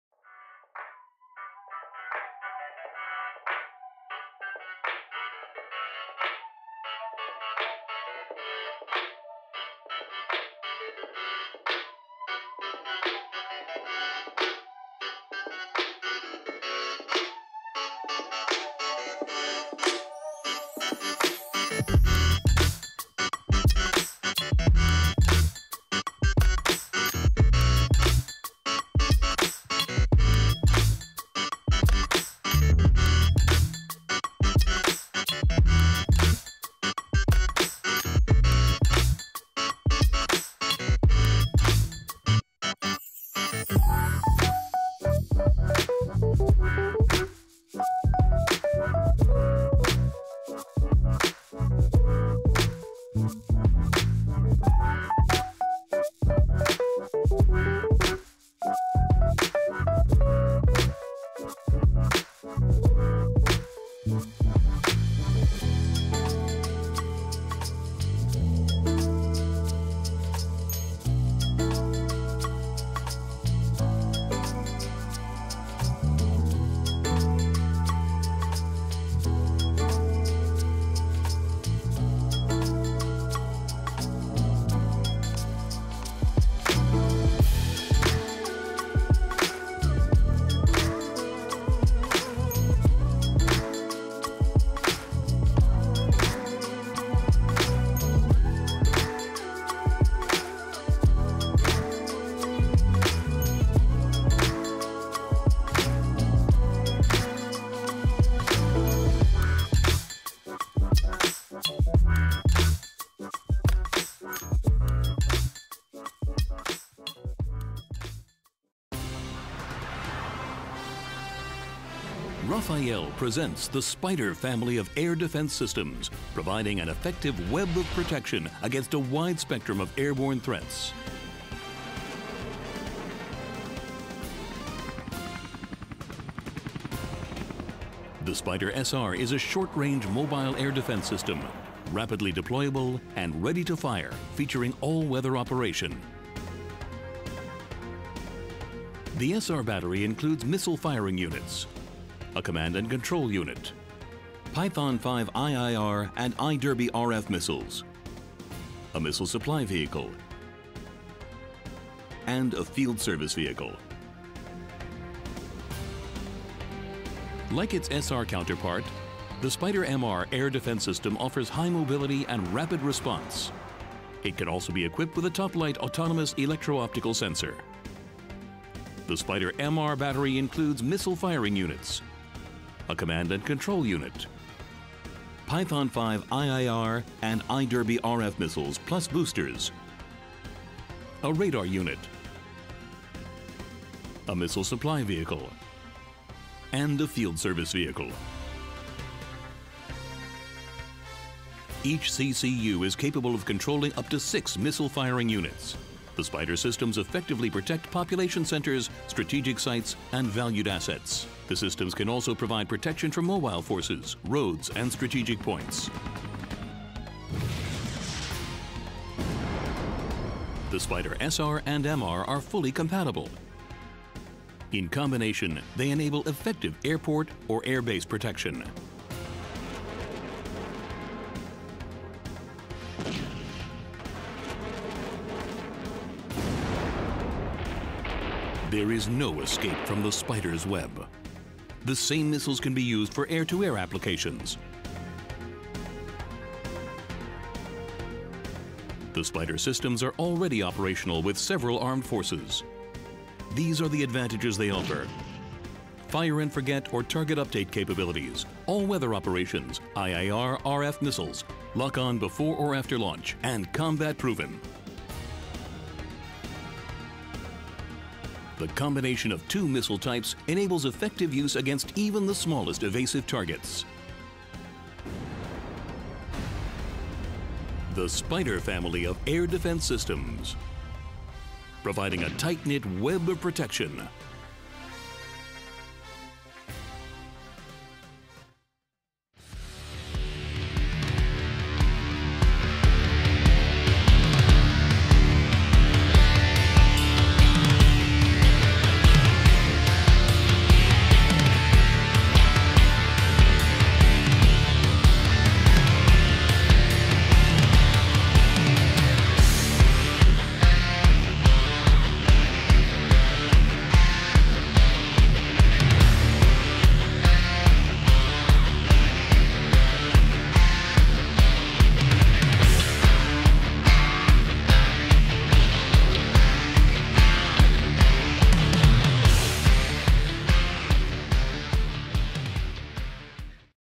Philippine Air Force incoming SPYDER AIR DEFENSE SYSTEM
End User: Philippine Air Force (960th Air and Missile Defense Group)

* Quantity: 3 batteries

* Modernization Phase: Horizon 2 phase of RAFPMP

* Project ABC: Php6,846,750,000.00

* Acquisition Mode: Government-to-Government (G2G) deal with Israeli Government

* Source of Funding: GAA Funds through AFP Modernization Program Trust Fund

* SARO Release:
      - SARO-BMB-D-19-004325 dated 04 June 2019 worth Php1,027,012,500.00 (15% Downpayment), released 06 June 2019

* Winning Proponent: Rafael Advance Systems Ltd. (Israel)

* Product for Delivery:
     - 3 batteries of Rafael SPYDER Air Defense System
     - Associated radar systems, command and control systems, support vehicles, and ammunition (Python 5 and Derby missiles)

* Contract Price: Php6,846,750,000.00

* Status: Notice of Award (NOA) released in favor of Rafael Advance Systems Ltd.